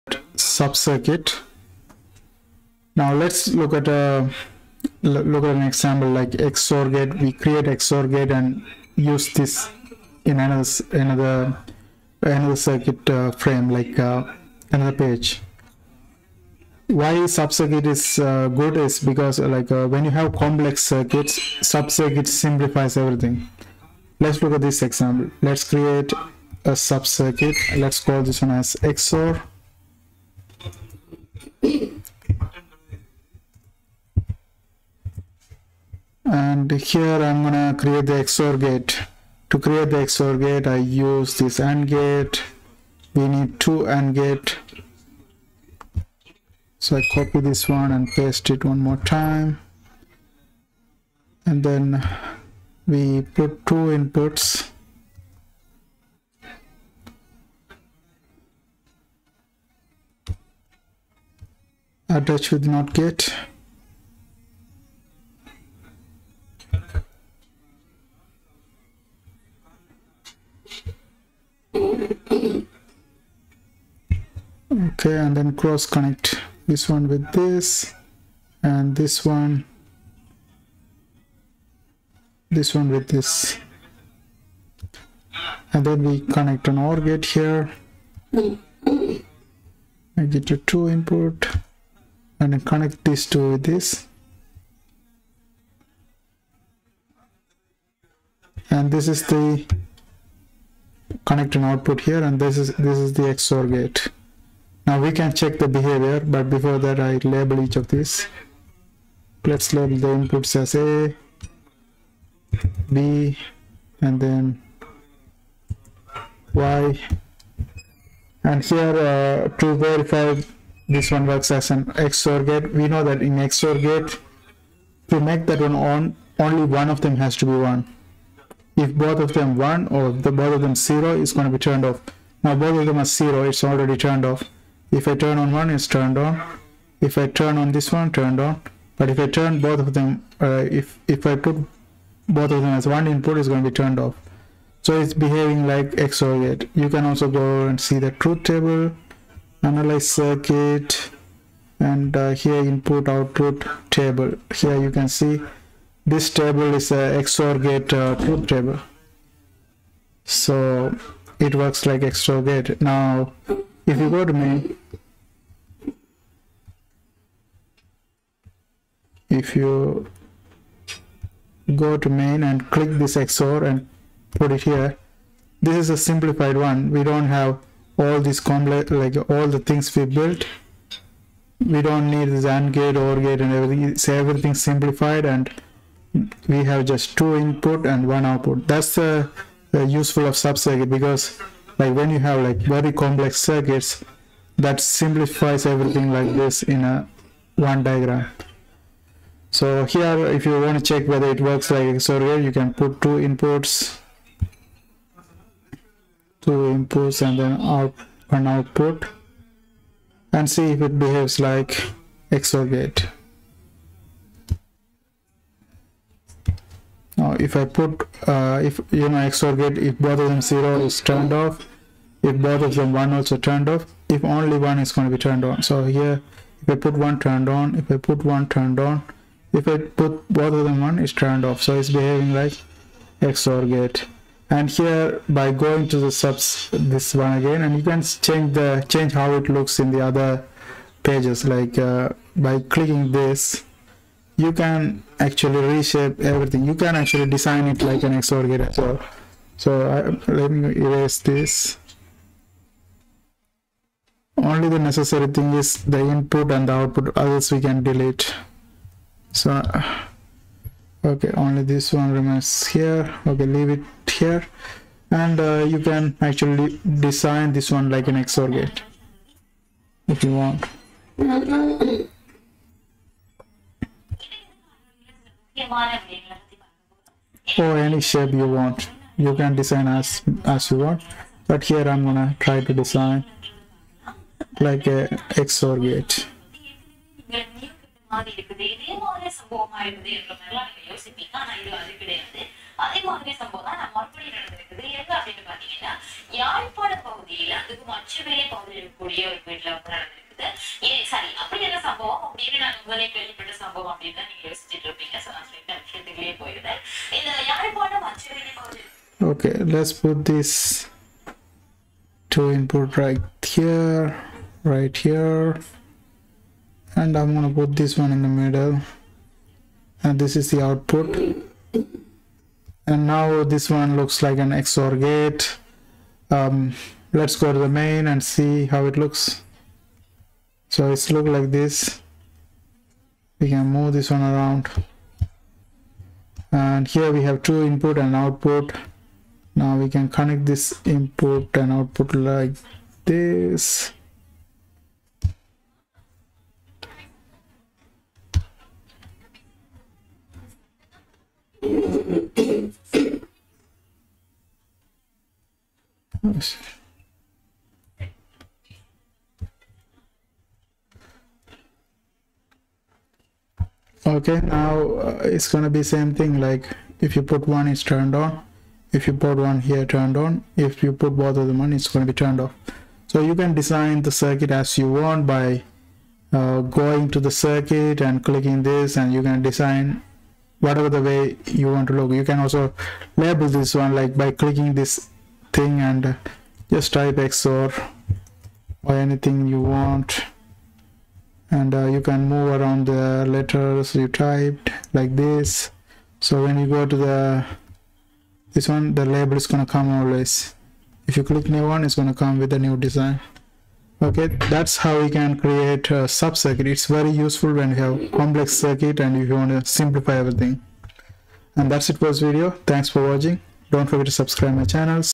Logisim Sub-circuits
In this screencast, we look at how to create and use sub-circuits in Logisim.